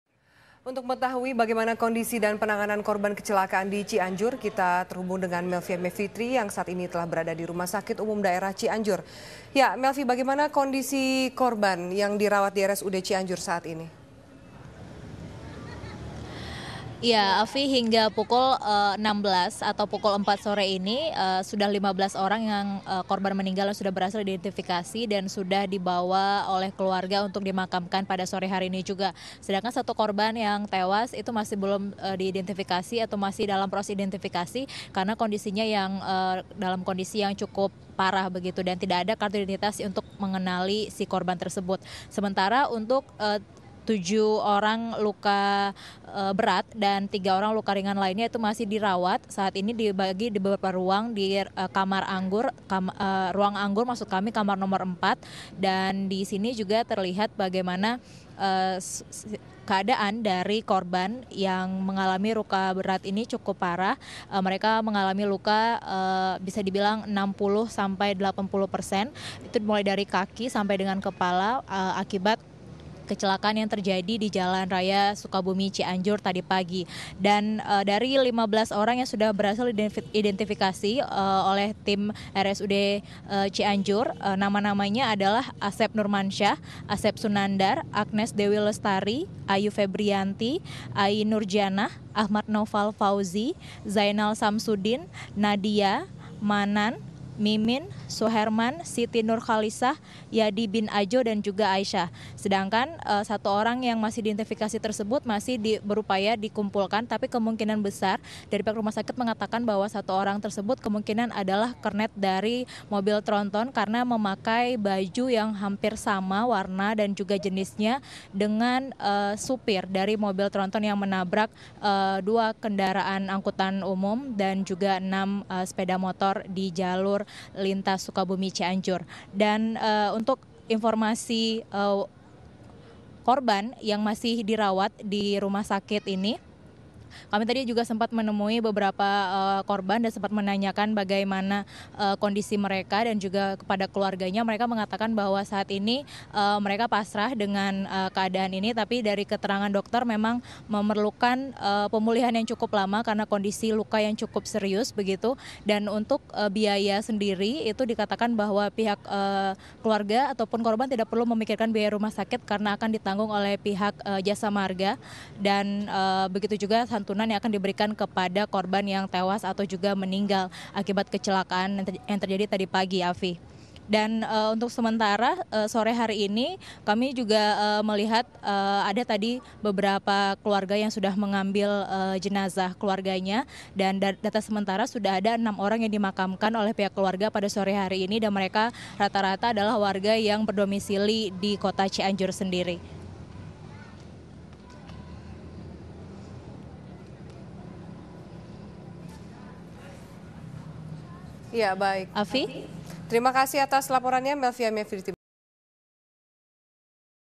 Satu Korban Kecelakaan Maut Cianjur Belum Terindentifikasi
Cianjur: Ruang jenazah Rumah Sakit Umum Daerah (RSUD) Cianjur, Jawa Barat, terus didatangi keluarga korban kecelakaan maut di Jalan Raya Cianjur-Sukabumi, Sabtu (23/2). Hingga pukul 16.00 WIB sudah 15 korban meninggal berhasil diindentifikasi.

Dari 15 korban, enam jenazah telah diambil keluarganya untuk langsung dimakamkan tadi sore. Mereka rata-rata merupakan warga yang berdomisili di Cianjur.

Hingga kini, masih satu korban belum berhasil diidentifikasi. Karena kondisinya sangat parah dan tidak ditemukan kartu identitas untuk mengenali korban. Diduga ia merupakan kernet truk tronton, karena mengenakan baju yang warna dan jenisnya hampir sama dengan yang dipakai sang sopir.

SEmentara itu, masih ada tujuh orang yang mengalami luka berat dan tiga orang luka ringan. Mereka saat ini masih dirawat di Ruang Anggur RSUD Cianjur. Korban rata-rata mengalami luka 60 persen hingga 80 persen, mulai dari kepala hingg kaki.

Keluarga korban mengaku pasrah. Dokter RSUD Cianjur menjelaskan, kemungkinan korban luka berat membutuhkan waktu agak lama untuk pulih karena luka yang cukup serius.

Pihak Jasa Marga dan Pertamina mengaskan, akan menanggung seluruh biaya rumah sakit dan juga memberuikan santunan pada keluarga korban yang meninggal. (Nin)